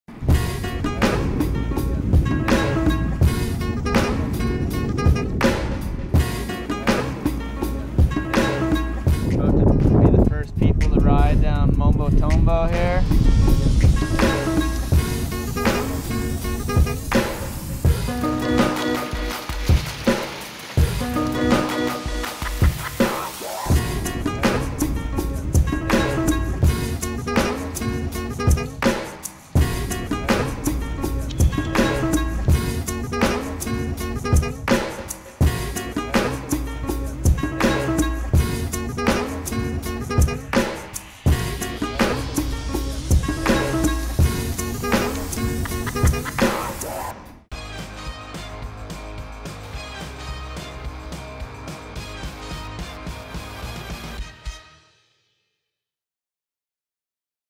Biking Down an Active Volcano | Volcanico
Last December, freeride mountain bike innovators Kurt Sorge, Nico Vink and Kyle Jameson embarked on a 10-day adventure riding some of the most unforgiving terrain on the planet, Nicaraguan volcanoes. This is the first time this type of fast, steep mountain biking has taken place on these peaks.